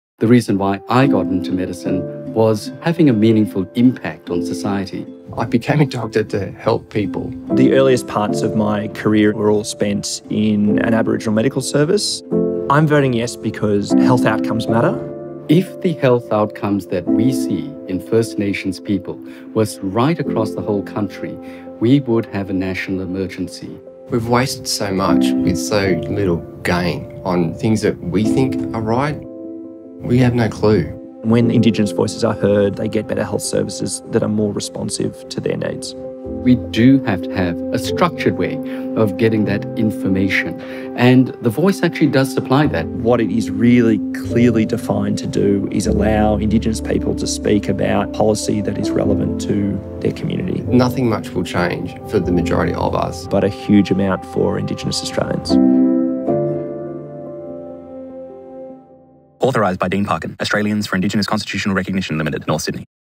Over 1000 Doctors Are Supporting Yes!
"If the health outcomes of First Nations people was right across the country, we would have a national emergency."

Over 1000 doctors have come together in support of a Voice to Parliament. Here's why.